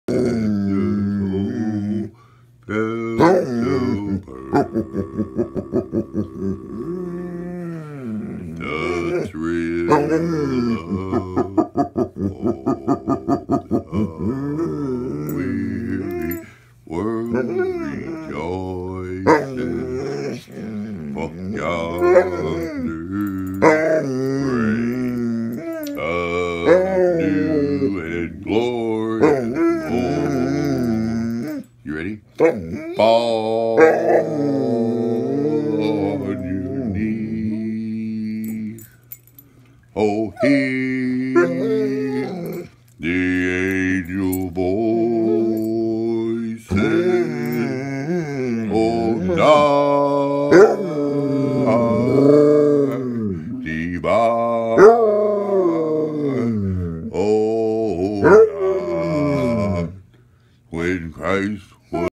Basset hound singing Ho Ho Ho
Sherman the basset hound singing Ho Ho Ho